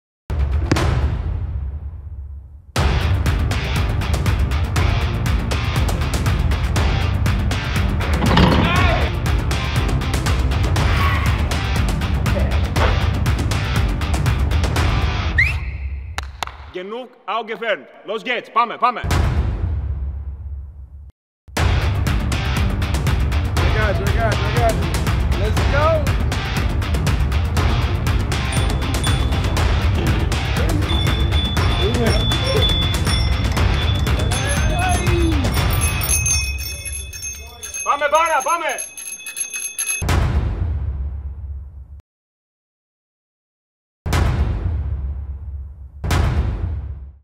STADTRADELN 2024: Basketball Löwen
Vom 1. bis zum 21. September macht auch Braunschweig wieder bei der Aktion @stadtradeln mit. 🚴‍♂️🚴‍♀️ Tretet in die Pedale und sammelt möglichst viele klimafreundlich gefahrene Kilometer mit dem Fahrrad, Rollstuhl oder Handbike. Auch die Basketball Löwen Braunschweig sind mit am Start! 🏀🦁

Mitmachen können alle, die in Braunschweig wohnen, arbeiten, zur Schule gehen, einem Verein oder einer Hochschule angehören. Meldet euch an unter 👉️ oder über die STADTRADELN-App.

❗️ Bei der Anmeldung könnt ihr euch für ein bestehendes Team anmelden oder ein neues registrieren.

🎬️ Stadt Braunschweig / Michaela Heyse